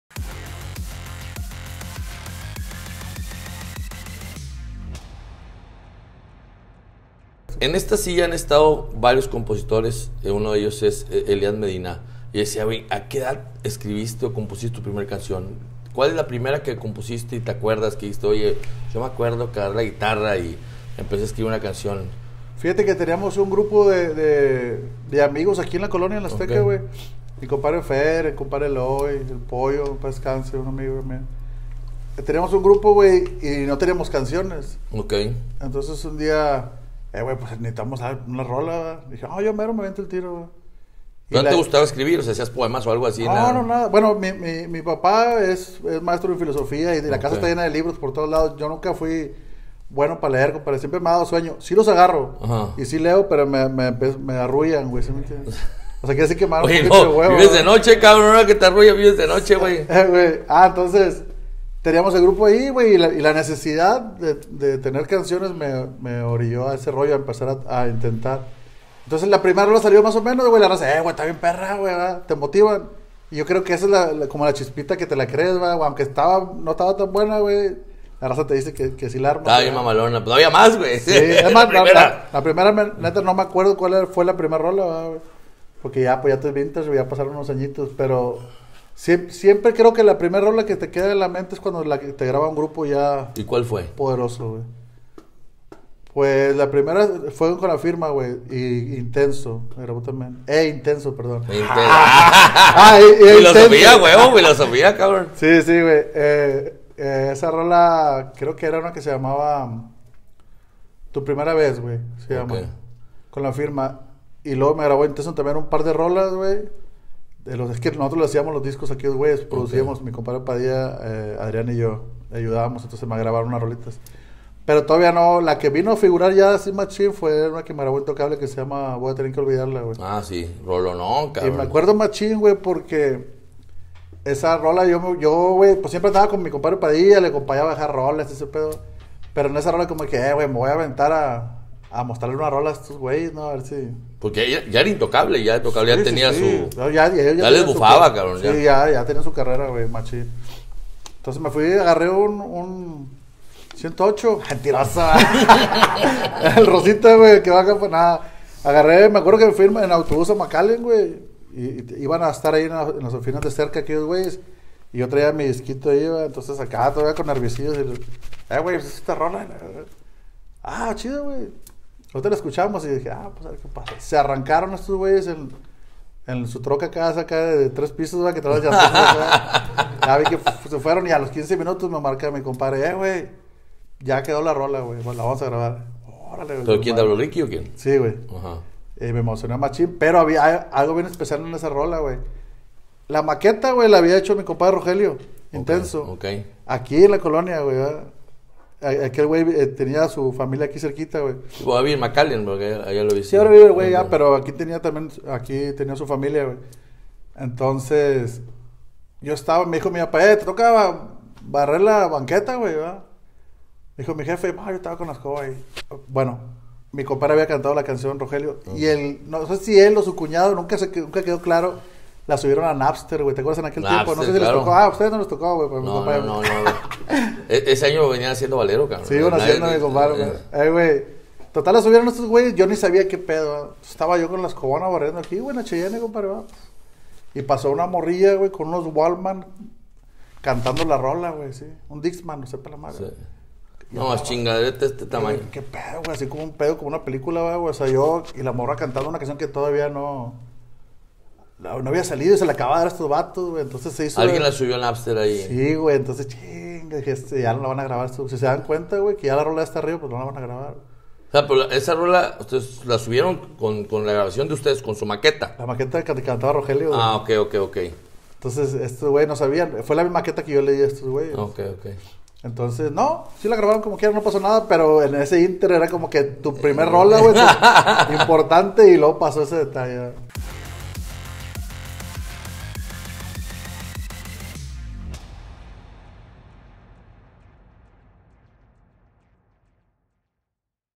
Las primeras canciones las grabó La Firma, Intocable e Intendo - Se Nos Hizo D Noche
Las primeras canciones que le grabaron a La Pantera fueron los grupos La Firma, Intocable y Grupo Intenso.